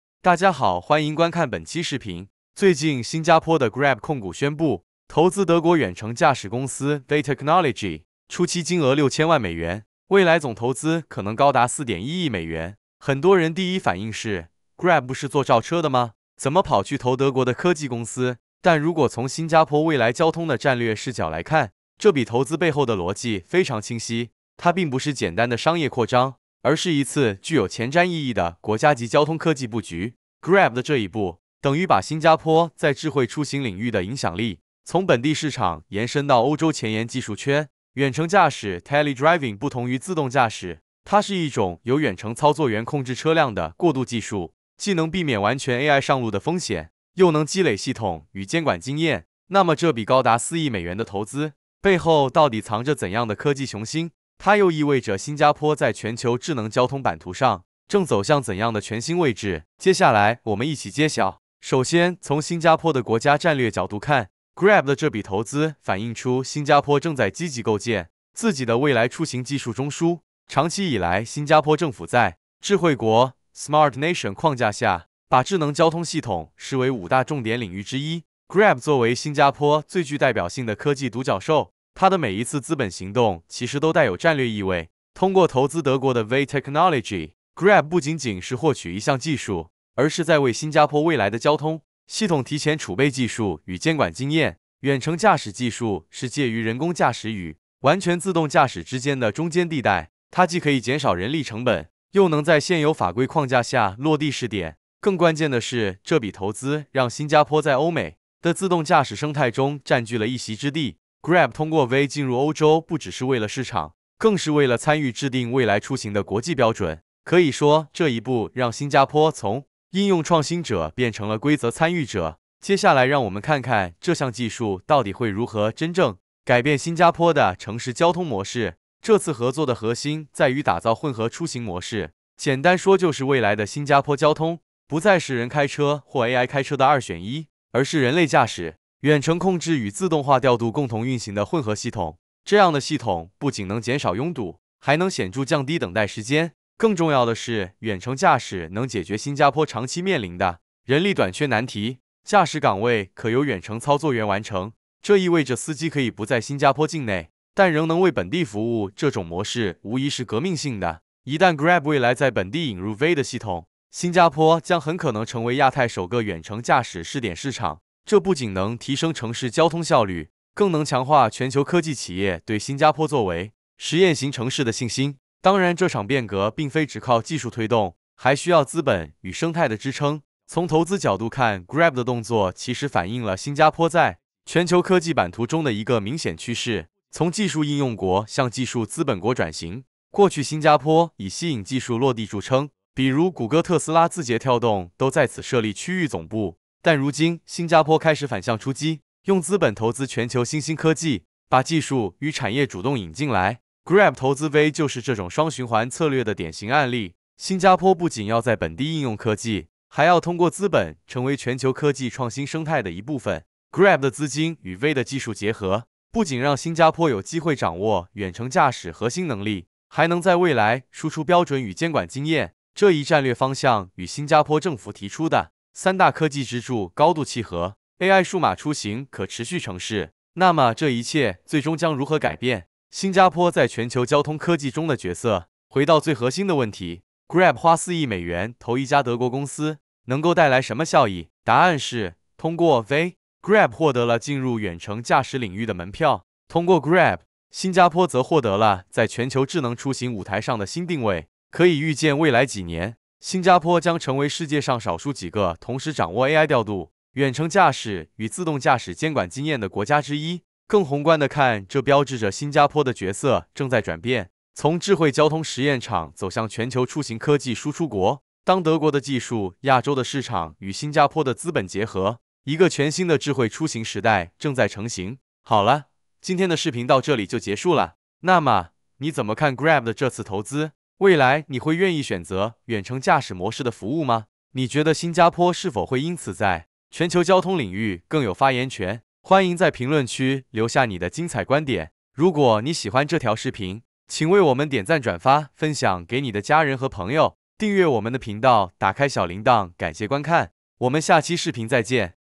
🚗 新加坡 Grab 出手4亿美元，投资德国远程驾驶公司 Vay，新加坡交通押注远程驾驶技术？
大家好，欢迎来到本期视频！今天我们将深入探讨新加坡代表性科技公司Grab，与德国远程驾驶公司Vay Technology之间的跨国合作，看这笔高达4亿美元的投资将如何影响新加坡全球交通科技布局。

⏱️ 视频章节：
00:00 Grab为何花4亿美元押注“远程驾驶”？
01:01 确立“未来出行技术中枢”的地位
02:08 推动“混合出行模式”，重新定义新加坡交通
03:00 资本与生态双循环:新加坡的科技投资逻辑
03:55 从“智慧交通实验场”走向“全球输出国”

💬 我们想听到你的声音！
你会对远程驾驶模式感兴趣吗？Grab这次投资是否能够提升新加坡在全球交通领域的话语权？欢迎在评论区畅所欲言！
📢 别忘了！